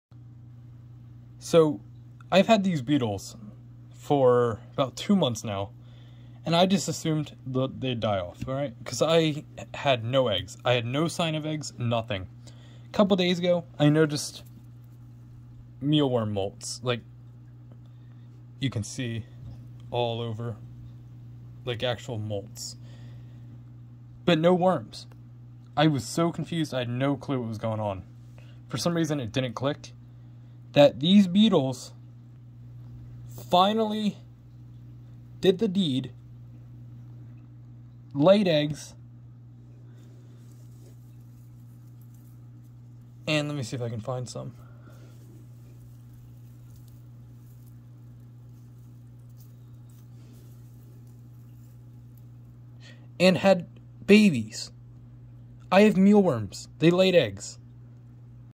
I guess my beetles laid eggs finally... because I'm starting to see mealworms!
I bought 200 mealworms for my Leopard Gecko in November, it was my first batch. I knew they would grow into Darkling Beetles eventually, but my plans were for them to lay eggs, and I'd move the eggs/worms into another container. Turns out, they didn't want to lay eggs... at least not until recently! I guess they sensed that I gave up trying to breed them, and decided to lay eggs to spite me!